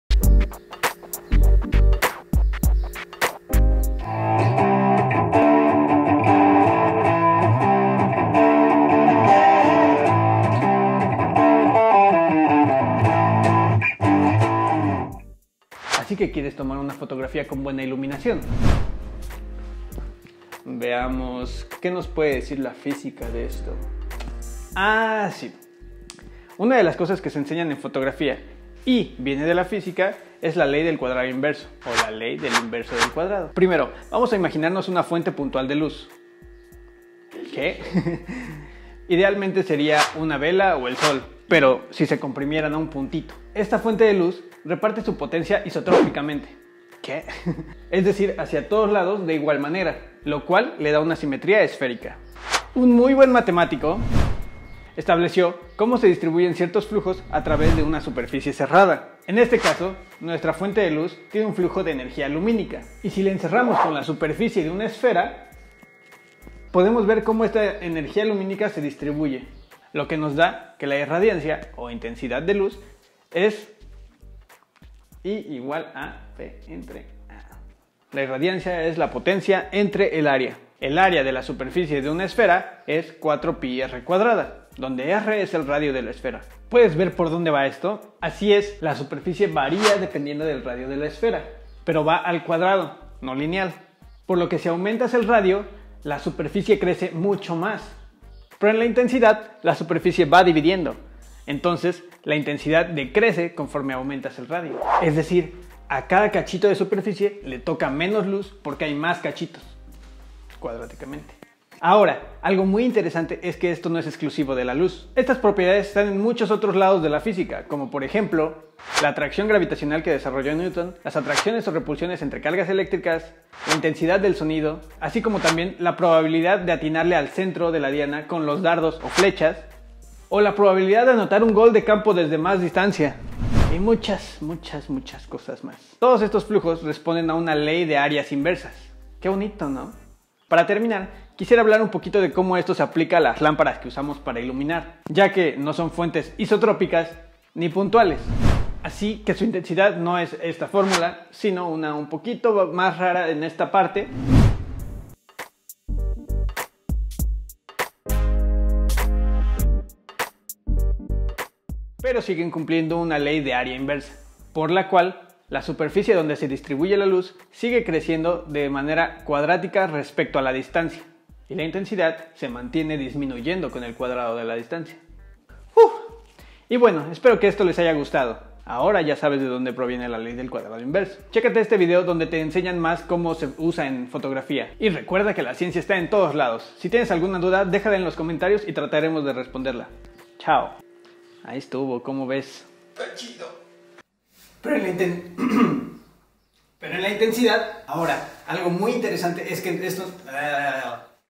¿Por qué hay una Ley del Cuadrado Inverso en fotografía? | Ciencillamente
Un amigo fotógrafo me pidió ayuda para completar una de sus clases

por la ayuda con este video.

Hay mucho más que se puede decir de este tema,
déjanos en los comentarios cualquier duda.

Y pueden postearnos dudas que quieran que expliquemos desde un punto de vista "100tifiko", o quizá quieran que hablemos de algún animalito, proceso químico, fenómeno natural... ustedes pidan!

:D ahora sí, sigan con sus vidas :D